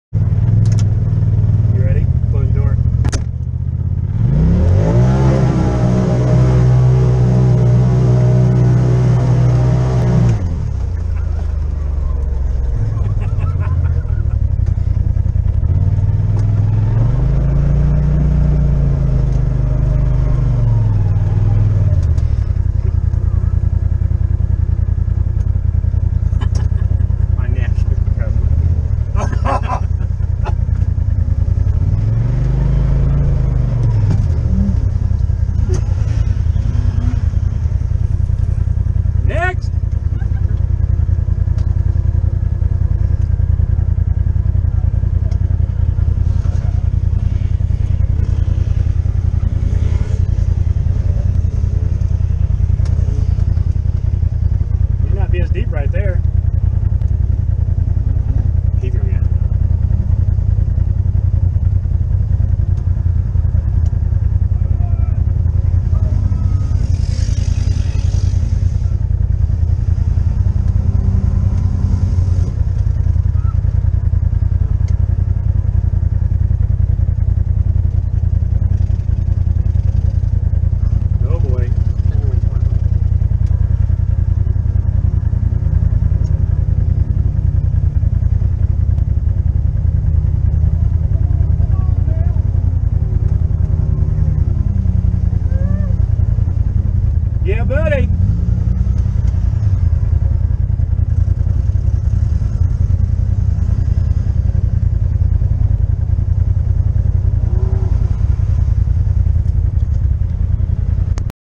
Interlake SRA Pond -- Trails 1C & 1D
Interlake group ride...The ponds where trails 1C and 1D meet.